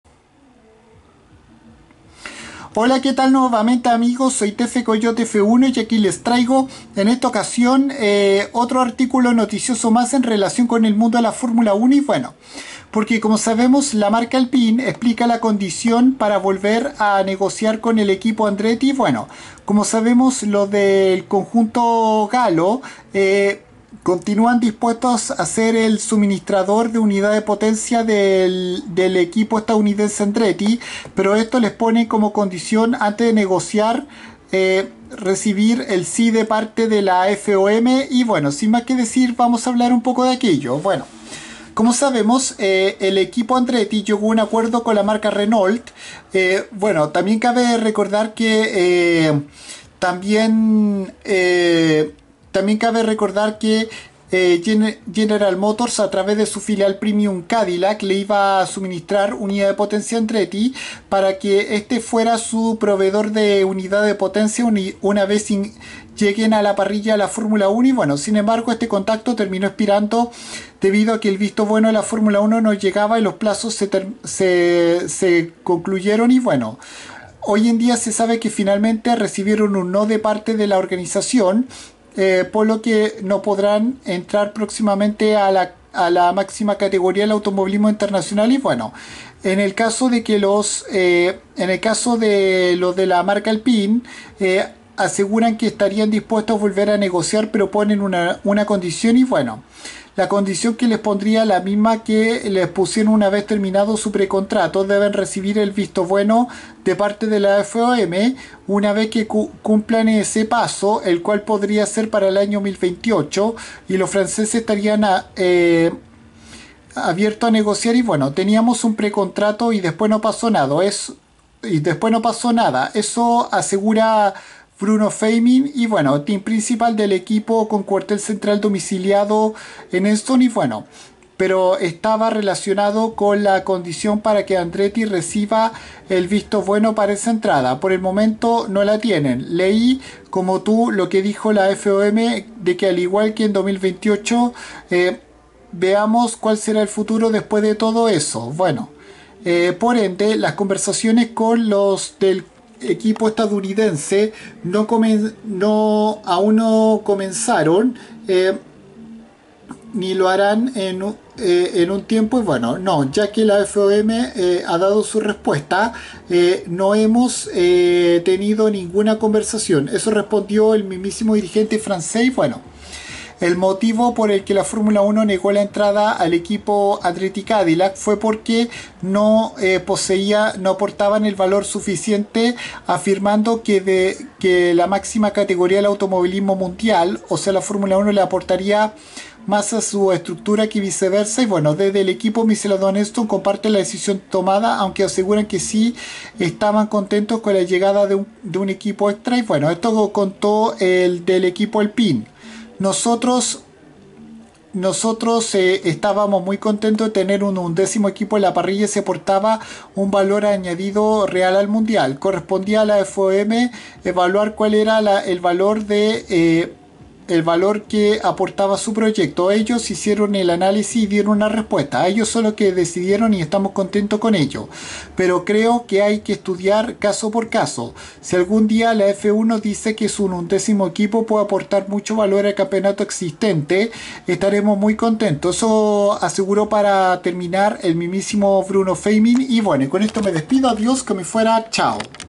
Alpine explica la condición para volver a negociar con el equipo Andretti
Ahora se habla de que los franceses puedan volver a motorizar a los del conjunto americano solo si reciban el "si" de parte de la FOM